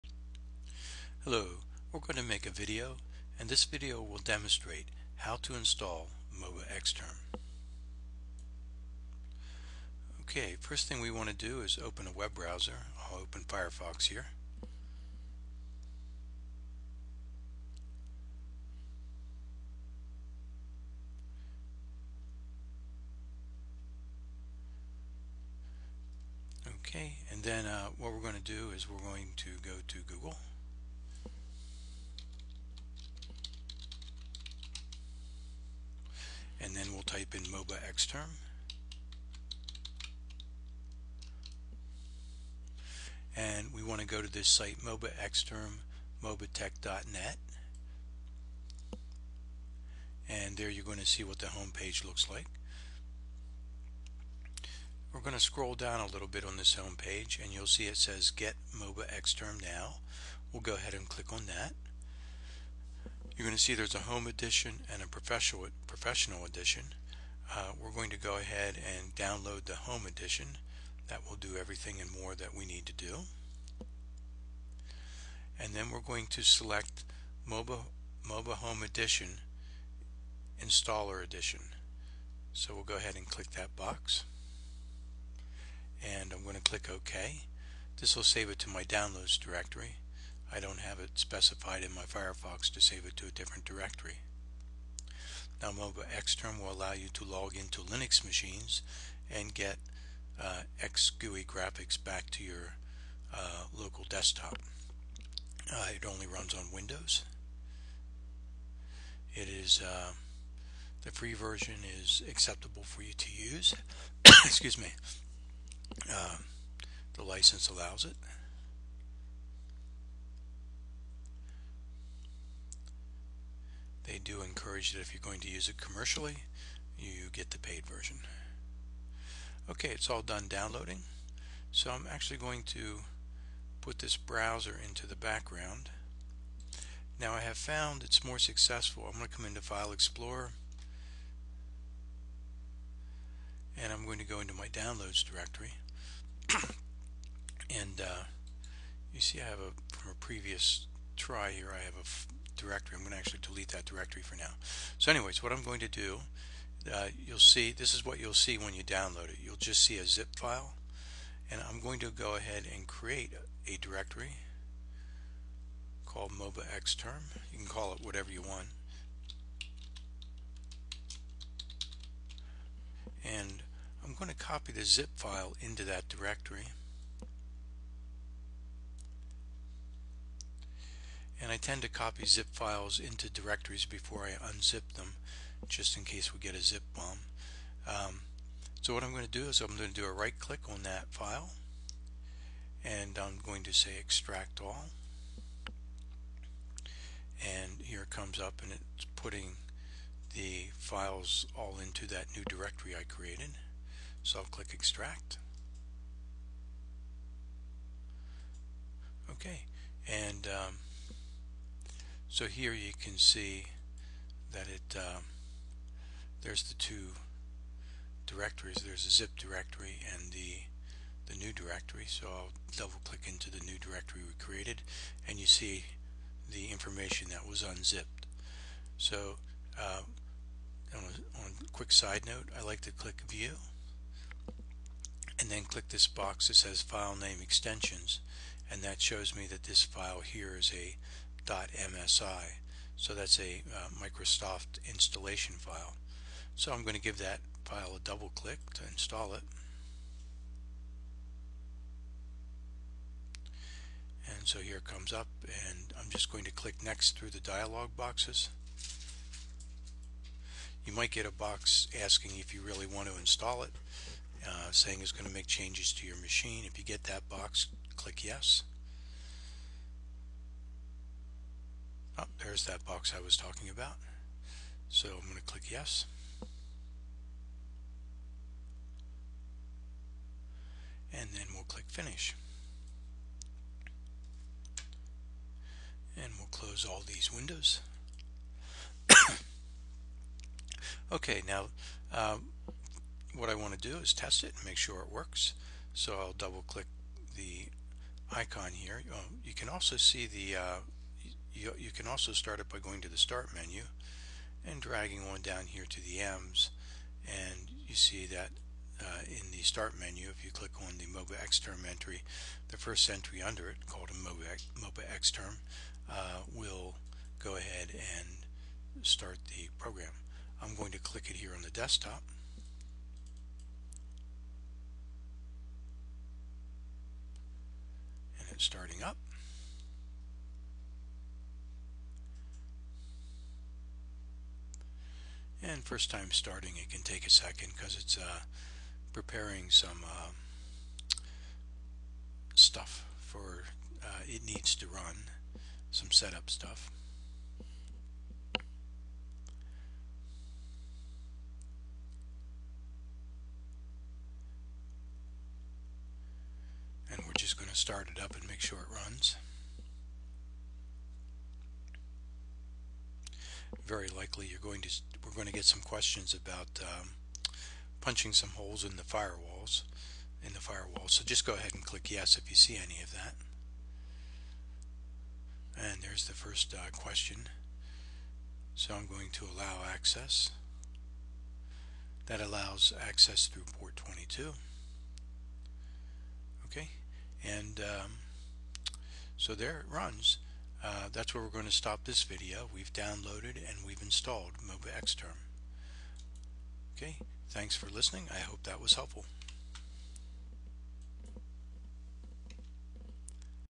03 30 2020 Install MobaXterm
This video demonstrates how to install MobaXterm on Windows 10